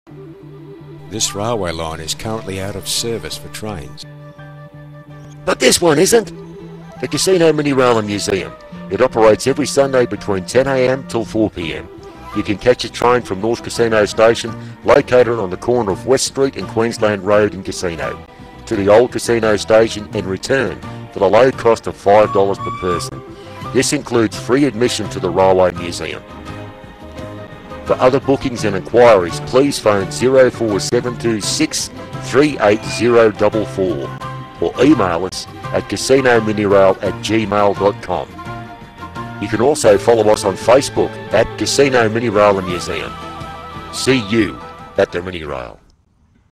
Casino Mini Rail
A short advertisement for the Casino Mini Rail & Museum. 2016